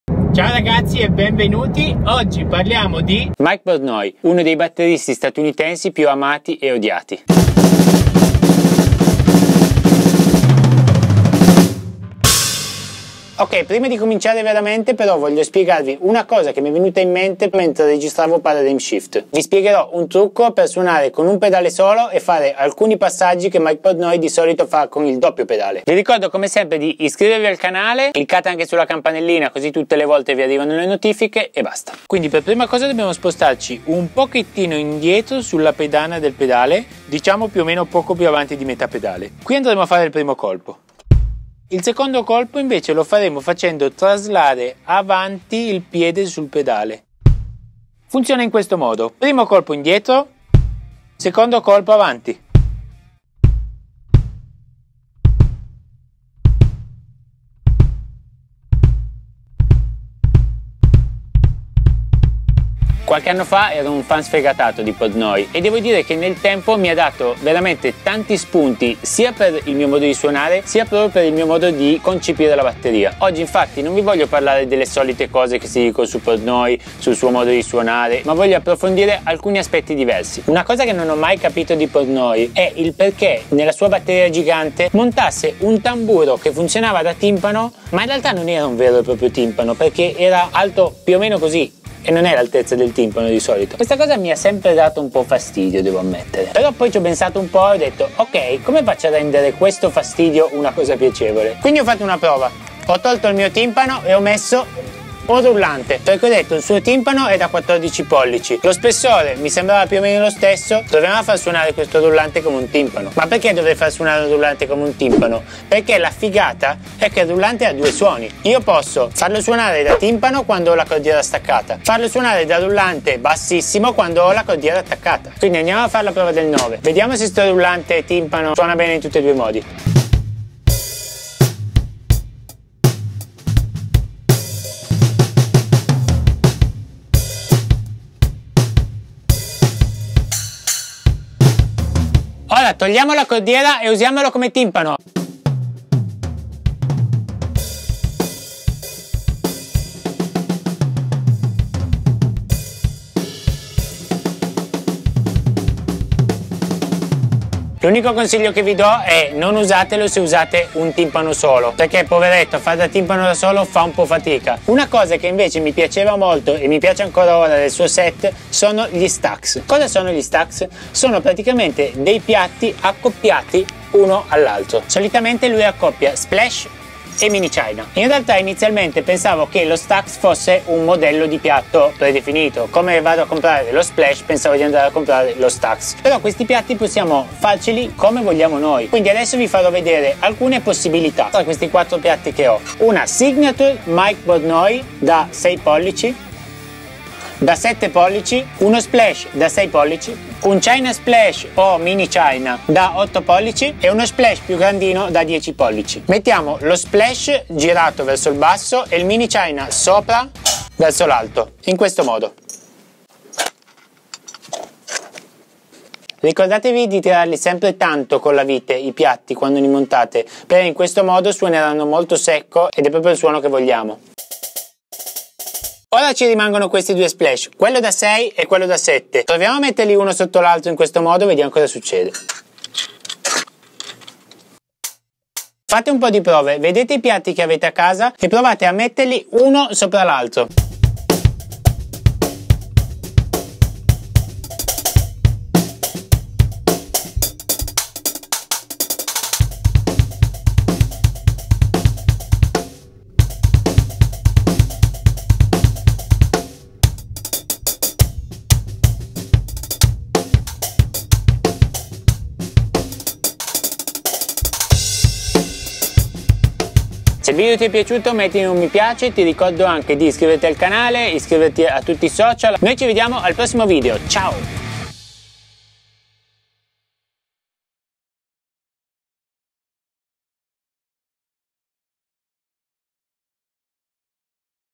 RUBIAMO a MIKE PORTNOY! Il DOPPIO delle possibilità con METÀ DELLO SFORZO
Oggi vediamo alcuni spunti che mi sono venuti in mente guardando Mike Portnoy! Non parleremo del suo stile ma di come poter rendere proprie alcune idee che possono essere utilizzate in qualsiasi genere!

Un video a settimana: tutti i martedì alle 11:30

♬ My gear:
Kits: